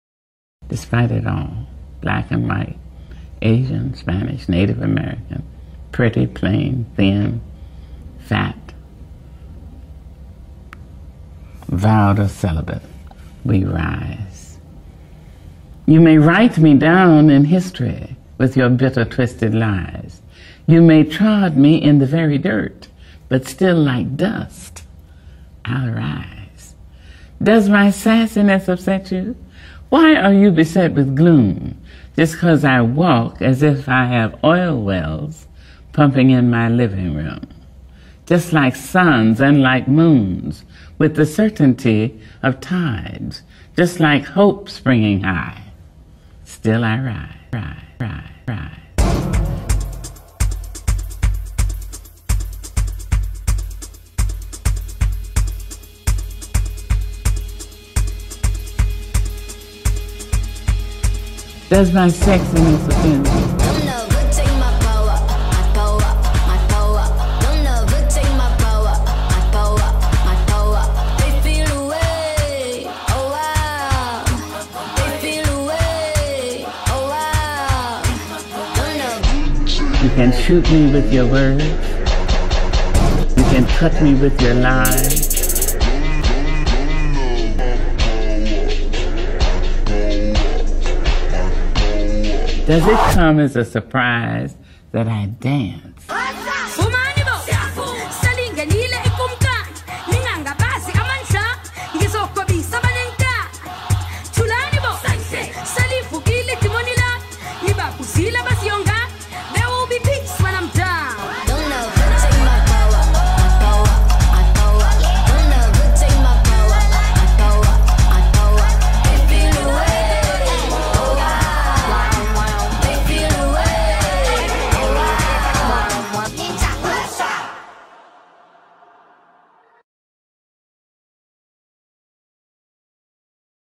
Duchess Division : WOMEN (Gold Medalist)
The 2020 Intercontinental Online Dance Championship hosted by Global Dance Supreme was held on 28 & 29 November .